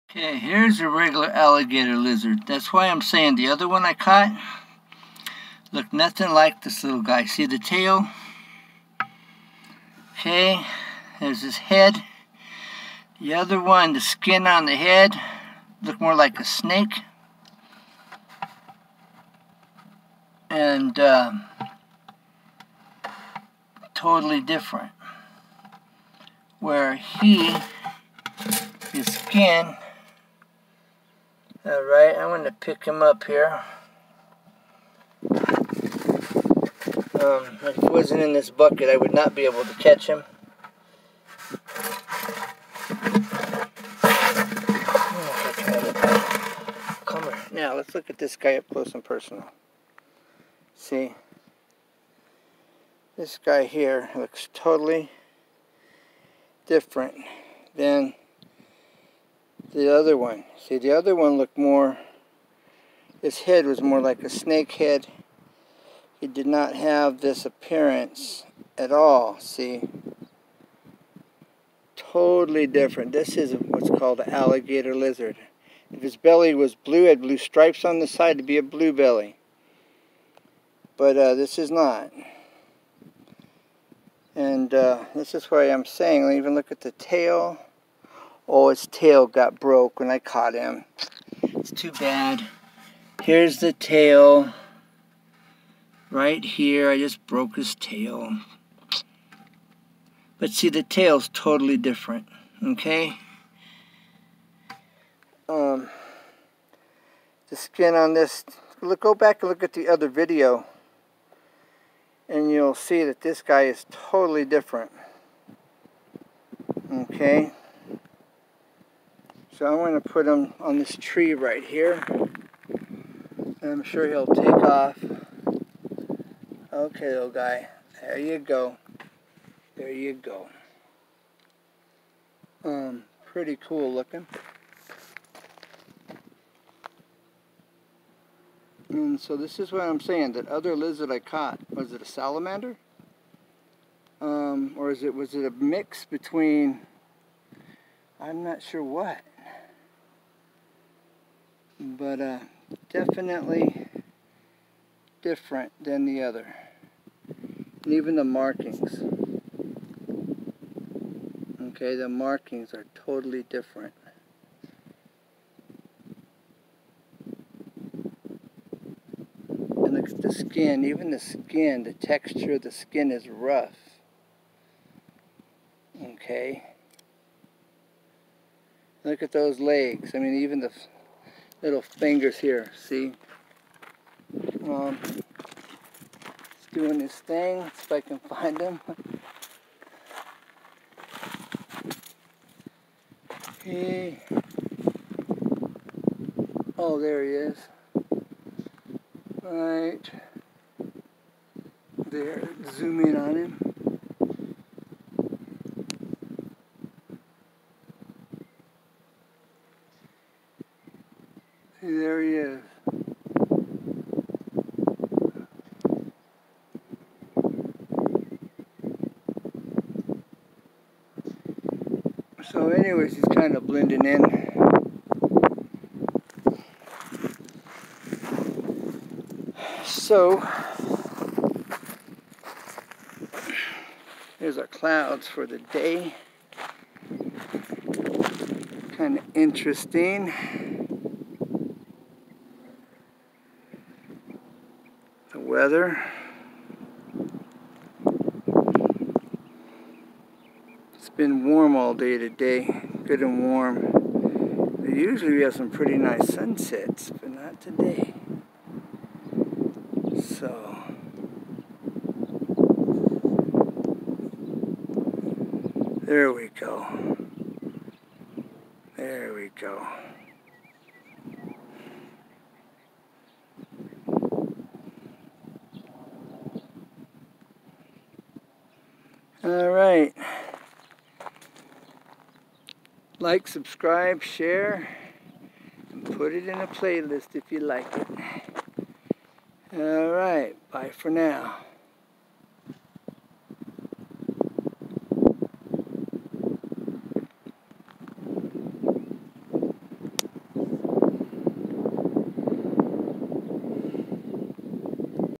" Alligator Lizard & My Skies "
Alligator lizard & my Skies; and this video I will be pointing out the differences between my last video with the salamander or the salamander mix cuz there's definitely a huge difference between the two types of lizards the one in this video and the lizard in the other video, are you uploaded the other day.
Salamander Lizard; or is it crossbred with something else?

Come on in. and check out this alligator lizard and let me know what you think.

Bye for now I look forward to hear from you God bless you, Ken